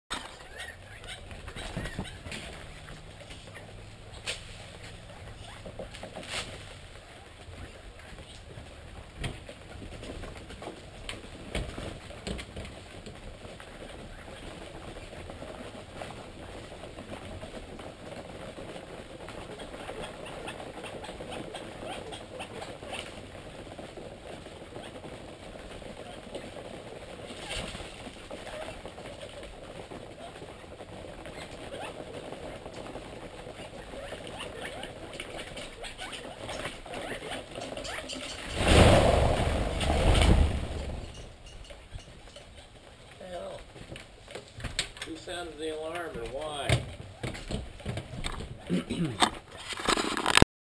Quail Cam - Alarm!
The birds of prey making my quail hella skittish.